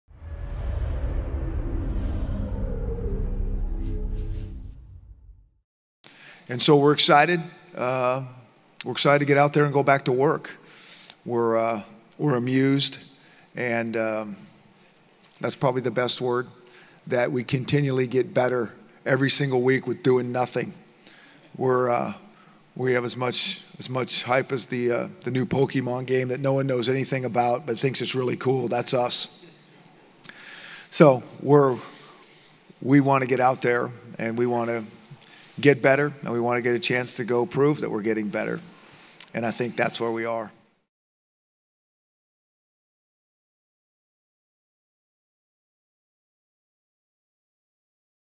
2016 Pac-12 Football Media Days: UW coach Chris Petersen likens his team to Pokemon Go craze
Washington head coach Chris Petersen likens his team's pre-season hype to the Pokemon Go craze while addressing reporters at Pac-12 Football Media Days in Hollywood.